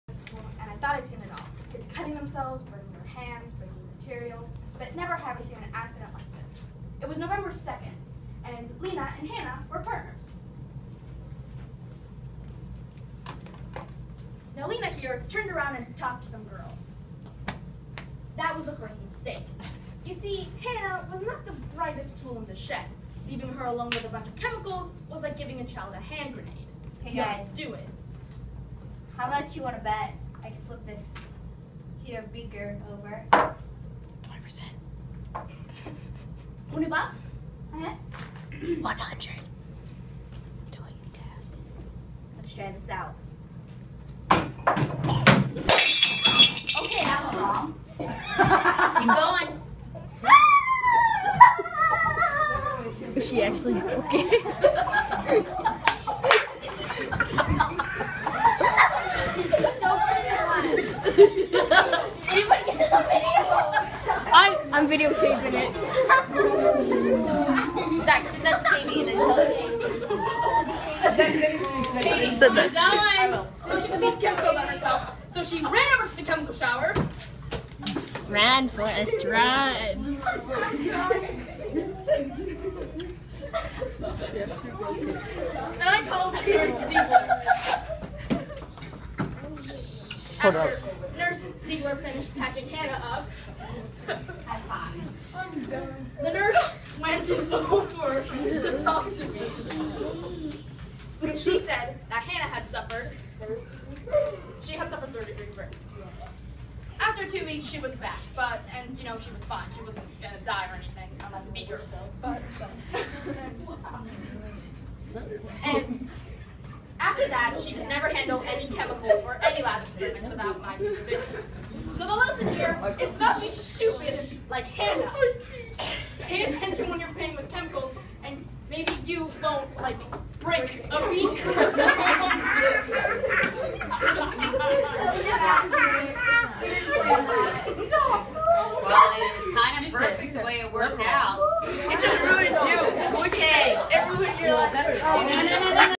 Beaker Breaker Accident
So in science we were doing safety skits of what not to do in the science lab, so a group made a script were a girl flips a water bottle and tips a beaker over , burning herself from the chemicals. The skit went wrong and then the beaker fell on the floor and shattered. The science teacher was really cool about it and she told her to keep on going. In my opinion they did a really good job in going on. Video credits: Zach Lindstrom